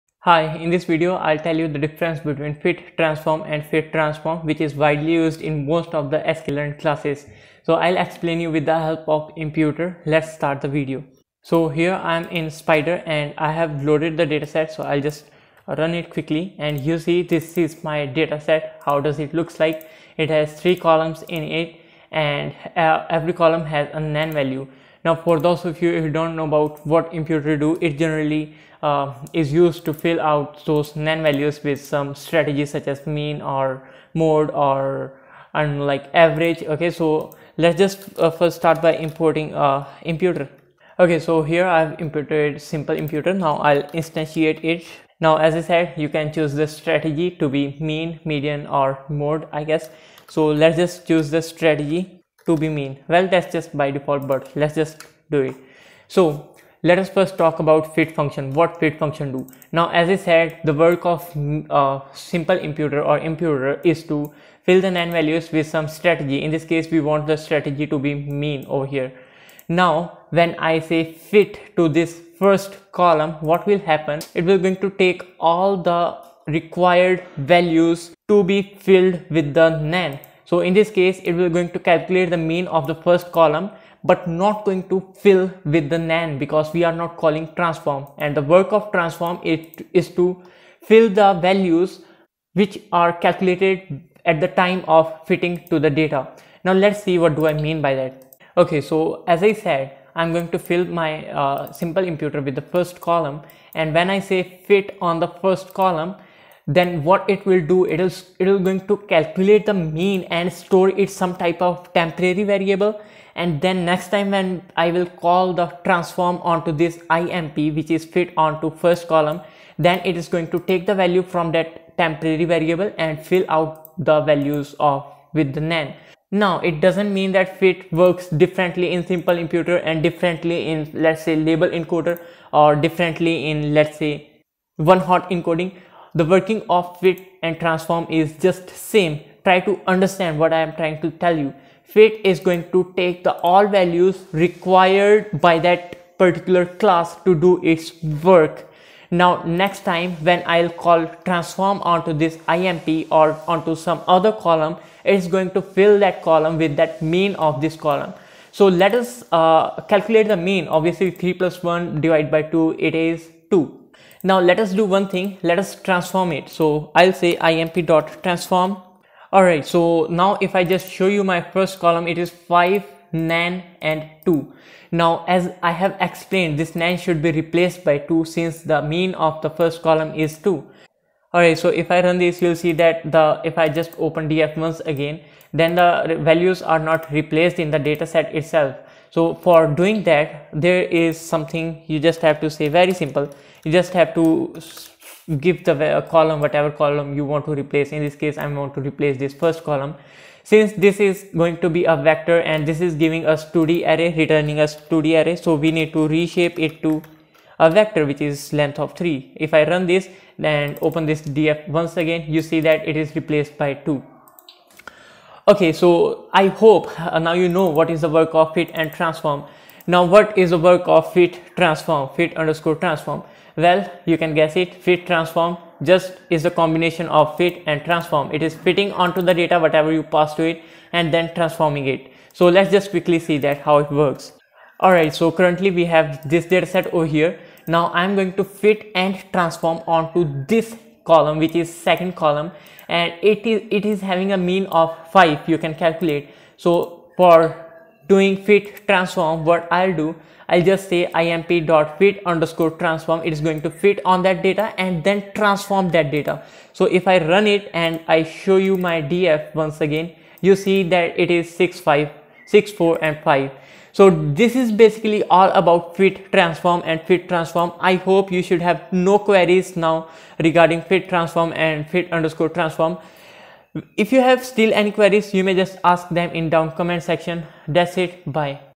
fit transform fit_transform | difference in fit and transform | sklearn | scikit learn
Hi, welcome to another video
In this video i tried clearing your doubts regarding fit transform and fit_transform which is bit confusing specially when you are starting with machine learning .
you can understand them by the help of this video I tried making this video as short and as informative as possible. I used simple example of SimpleImputer or Imputer which is widely used to fill NaN values.
If you still have any queries make sure to ask them in comment section or any of below contact link.

FOR CONTACTING ME FOLLOW ANY OF THE LINK

Make sure to Like if you found it helpful.

fit transform fit_transform